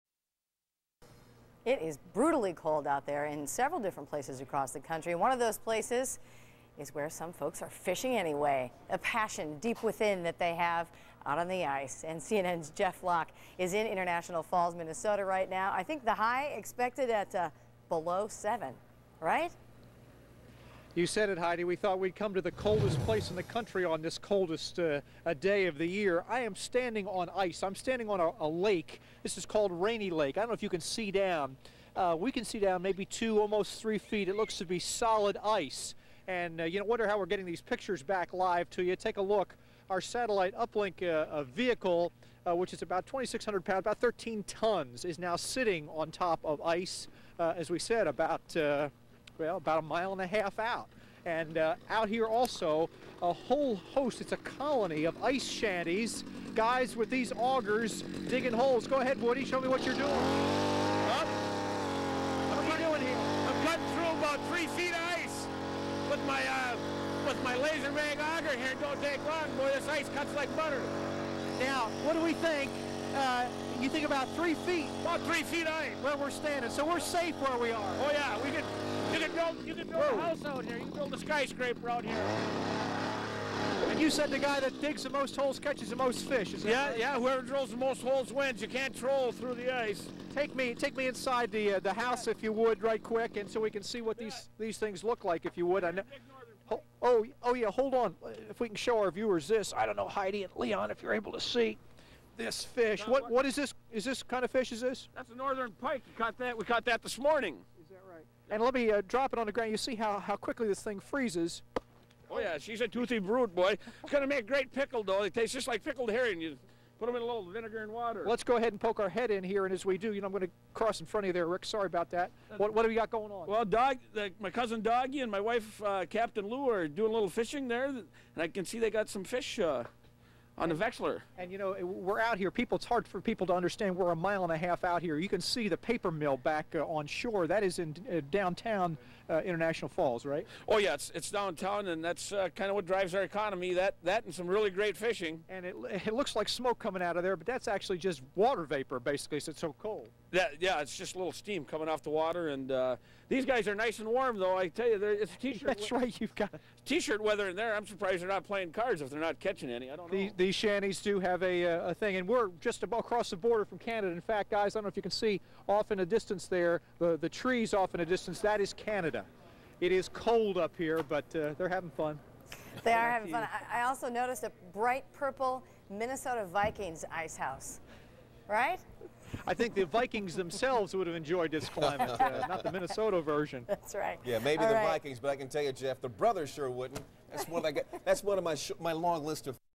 Woody's Fairly Reliable Guide Service
CNN - Brrrrutal cold, ice fishing Rainy Lake, MN